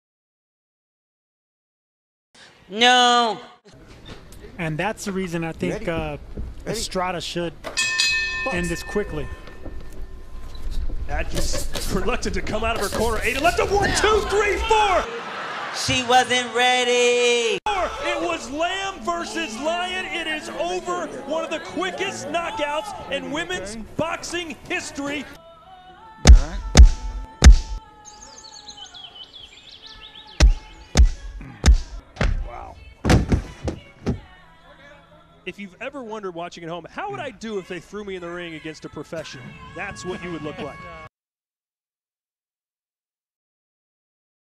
Seniesa Estrada Vs Miranda Adkins (7 Sec Domination)
Estrada beat her so bad her shorts went from "MOM to WOW"!
Adkins NEVER should have been in that ring with Estrada.  5.0 vs 18.0